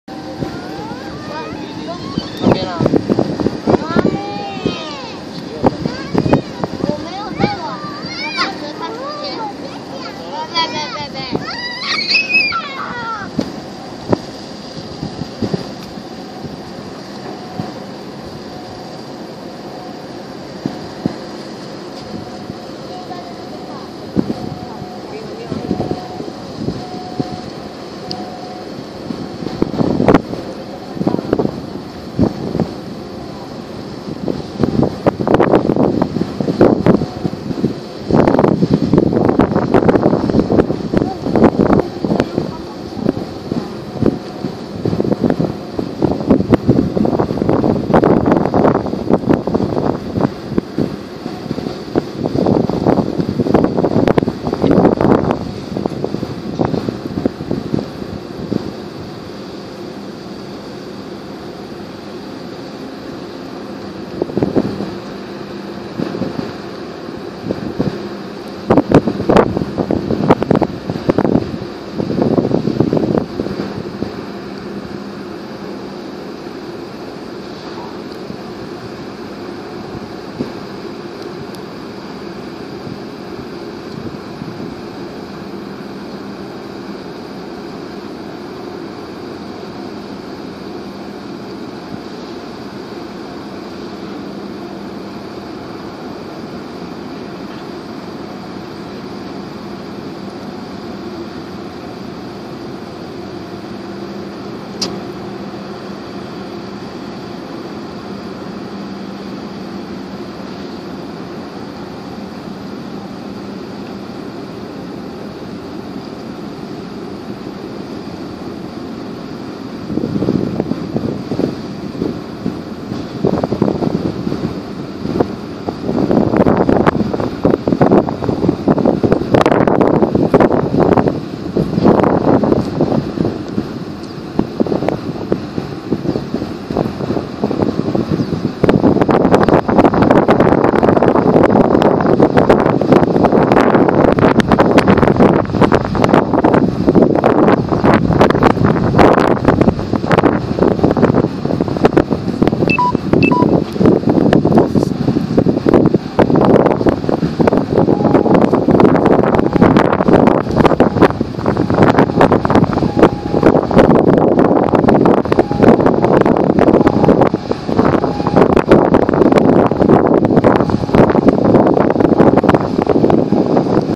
110/9/4 萬海海春WAN HAI 505首次記錄 第107航次 二港進停64號萬海航運碼頭 船籍新加坡 航線Asia - South America I Service (AS1)
ATD: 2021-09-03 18:53 LT (UTC +8) HONG KONG HK,HKG
前一港:Hong Kong (9/2-9/3) 次一港:Ensenada, Baja California (9/13-9/14)

IMO 9294874
Callsign S6AV3
Flag (Registration) Singapore
Gross Tonnage42894
Deadweight (t)52146
Length (m)268.8
Beam (m)32.2
TEU 4333
Built (year) NOV 2005
Builder CHINA SHIPBUILDING CORPORATION, KAOHSIUNG SHIPYARD.